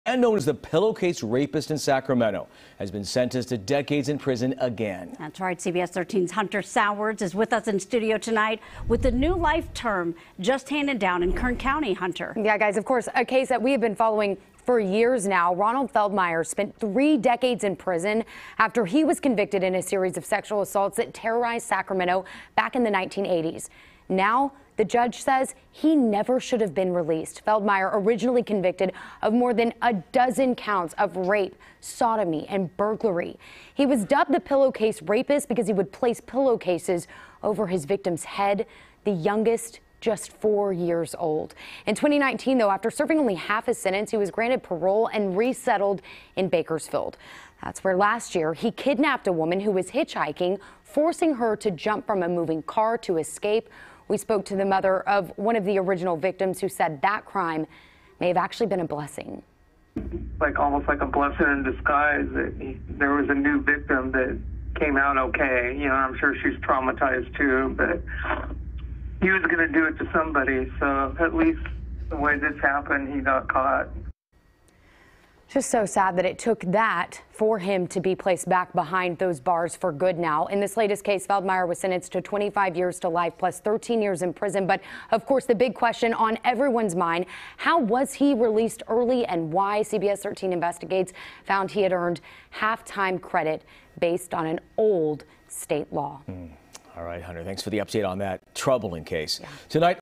Man known as "Pillowcase Rapist" sentenced to decades in prison
The man known as the "Pillowcase Rapist" in Sacramento has been sentenced to decades in prison again.